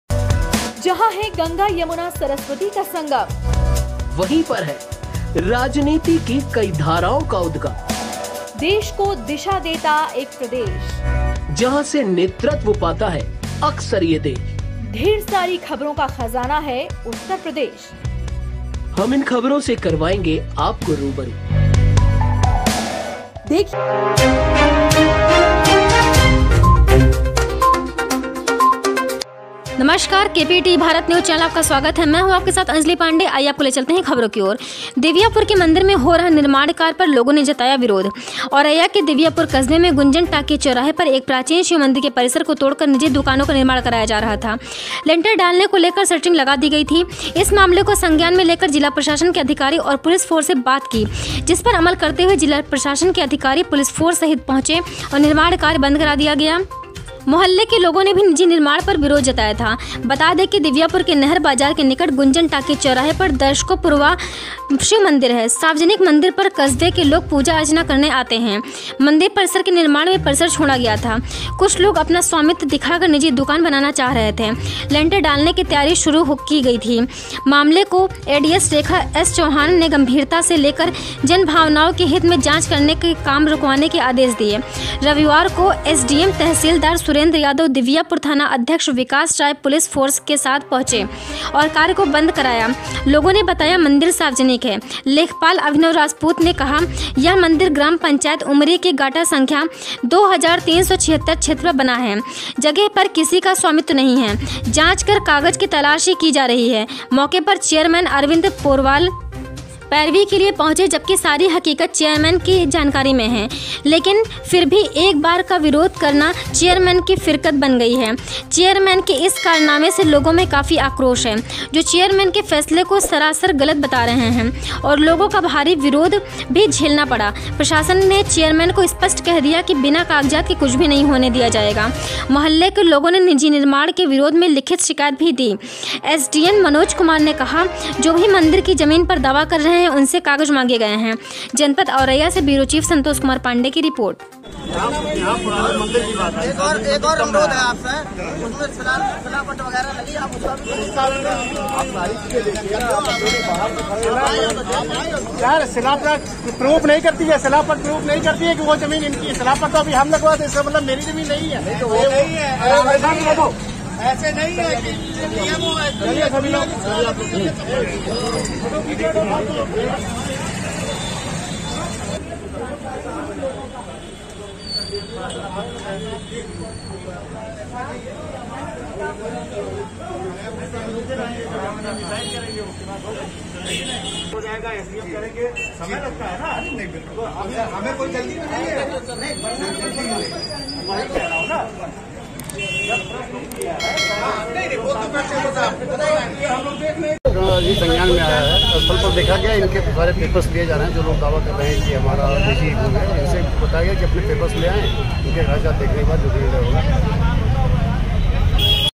दिबियापुर के मंदिर में हो रहा निर्माण कार्य पर लोगों ने जताया विरोध
औरैया

संवाददाता संतोष कुमार पांडे औरैया

औरैया के दिबियापुर  कस्बे में गुंजन टॉकीज चौराहे पर एक प्राचीन शिव मंदिर के परिसर को तोड़कर निजी दुकानों का निर्माण कराया जा रहा था लेंटर डालने को लेकर शटरिंग लगा दी गई थी, इस मामले को संज्ञान में लेकर जिला प्रशासन के अधिकारी और पुलिस फोर्स से बात की जिस पर अमल करते हुए जिला प्रशासन के अधिकारी पुलिस फोर्स सहित पहुंचे और निर्माण कार्यबंद करा दिया मोहल्ले के लोगों ने भी निजी निर्माण पर विरोध जताया था बता दें कि दिबियापुर के नहर बाजार के निकट गुंजन टॉकीज चौराहे पर दशकों पुराना शिव मंदिर है सार्वजनिक मंदिर पर कस्बे के लोग पूजा अर्चना करने आते हैं मंदिर परिसर के निर्माण में परिसर छोड़ा गया था कुछ लोग अपना स्वामित्व दिखाकर निजी दुकान बनाना चाह रहे थे लेंटर डालने की तैयारी शुरू हो गई थी मामले को एडीएम रेखा एस चौहान ने गंभीरता से लेकर जन भावनाओं के हित में जांच और काम रुकवाने के आदेश दिए रविवार को  एसडीएम तहसीलदार सुरेंद्र यादव दिबियापुर थाना अध्यक्ष विकास राय पुलिस फोर्स के साथ पहुंचे और कार्य को बंद कराया लोगों ने बताया मंदिर सार्वजनिक है लेखपाल अभिनव राजपूत ने भी कहा यह मंदिर ग्राम पंचायत उमरी के गाटा संख्या 2376 क्षेत्र पर बना है जगह पर किसी का स्वामित्व नहीं है! जांच कर कागज की तलाश की जा रही है! मौके पर चेयरमैन अरविंद पोरवाल पैरवी के लिए पहुंचे जबकि सारी हकीकत चेयरमैन की जानकारी में है लेकिन फिर भी एक बार का विरोध करना चेयरमैन की फिरकत बन गई है चेयरमैन के इस कारनामे से लोगों में काफी आक्रोश है जो चेयरमैन के फैसले को सरासर गलत बता रहे हैं और लोगों का भारी विरोधभी झेलना पड़ा प्रशासन ने चेयरमैन को स्पष्ट कह दिया कि बिना कागजात के कुछ भी नहीं होने दिया जाएगा मोहल्ले के लोगों ने निजी निर्माण के विरोध में लिखित शिकायत भी थी एसडीएम मनोज कुमार ने कहा जो भी मंदिर की जमीन पर दावा कर रहे हैं!उनसे कागज मांगे गए हैं!